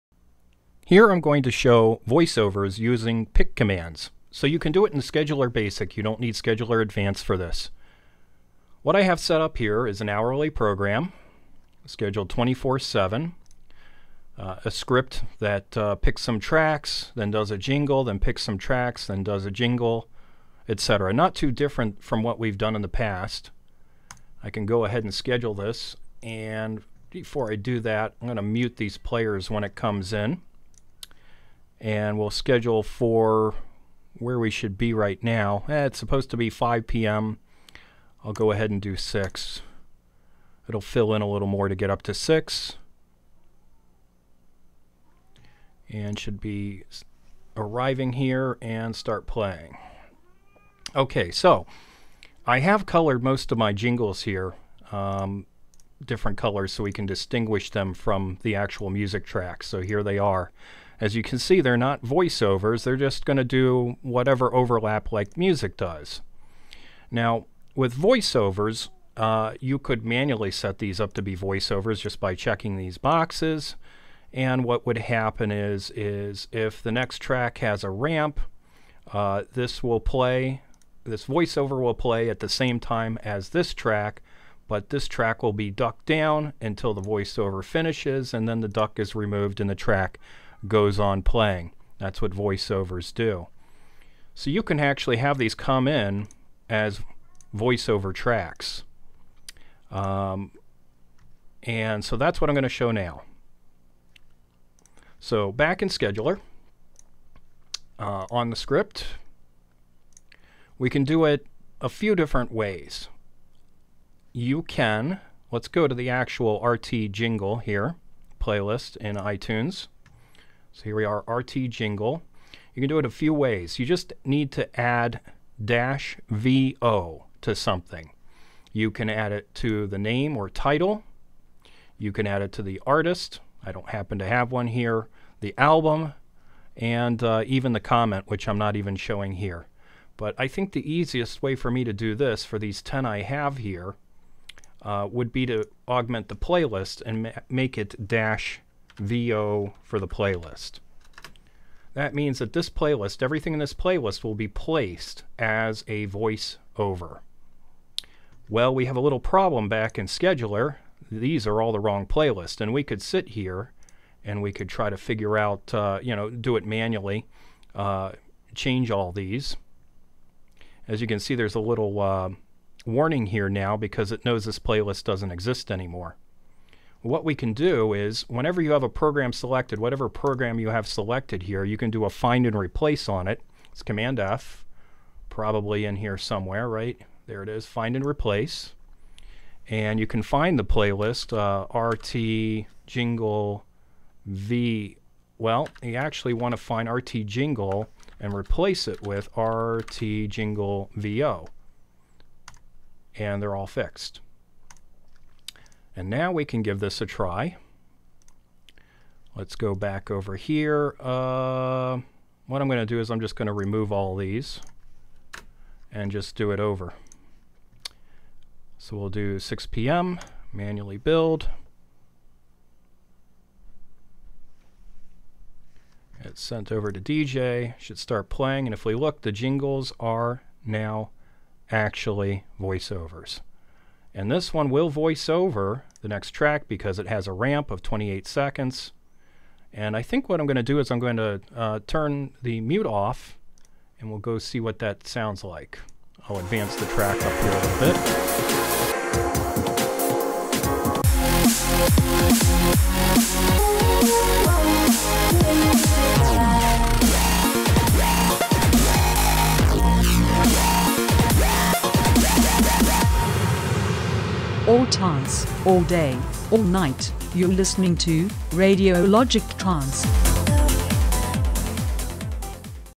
14. Radiologik Scheduler Voiceovers using picks
Use the simple pick command to pick from a playlist and turn any track into a voiceover.